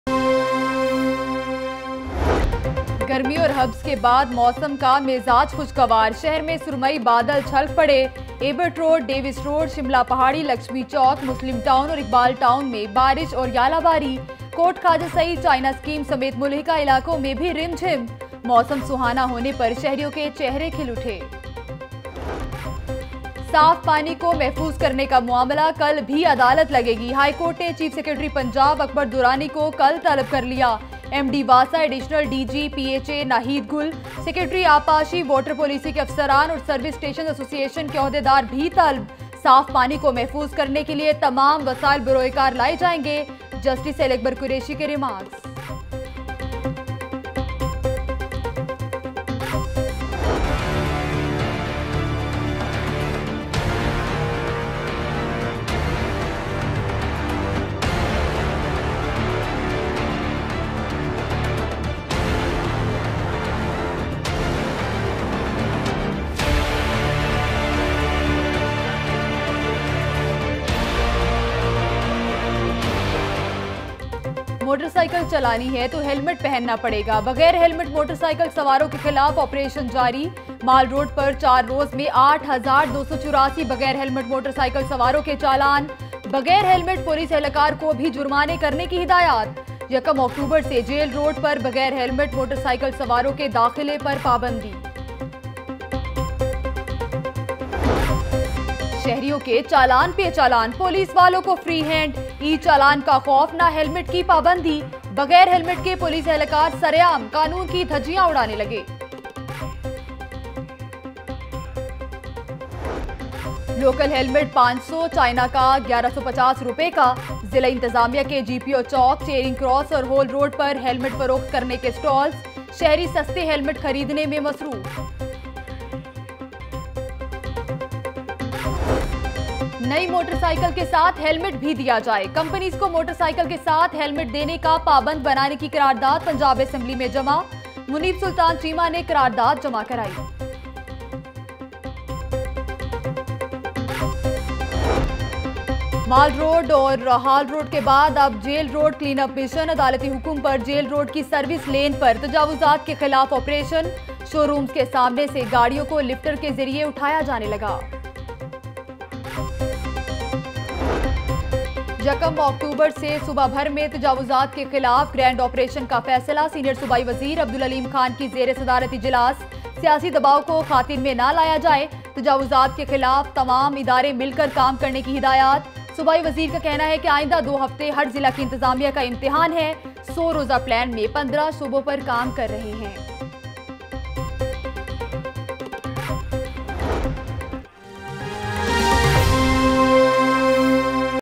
04 PM Headlines | Lahore News HD | 28 September 2018
Watch news for One dies in firing at Lohari - Anti car lifting staff's big action - CM Punjab orders to suspend officers involved in Model Town massacre - Unveil the culprits behind Model Town massacre, asks Tahir ul Qadri - One lac traffic violations recorded on daily basis - Hamza Shehbaz hopeful for a clean sweep in By polls -  Pakistan team announces 17 players squad for test series against Australia and much more.